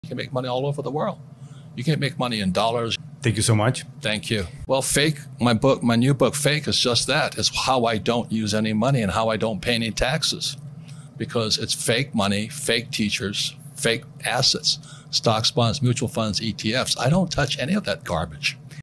Rich Dad: ETFs, Bonds, Mutual Funds I don't touch any of that garbage
📖 Subtitles:
You can make money all over the world You can't make money in dollars Thank you so much Thank you Well fake my book my new book fake is just that is how I don't use any money and how I don't pay any taxes because it's fake money fake teachers fake assets stock bonds mutual funds ETFs I don't touch any of that garbage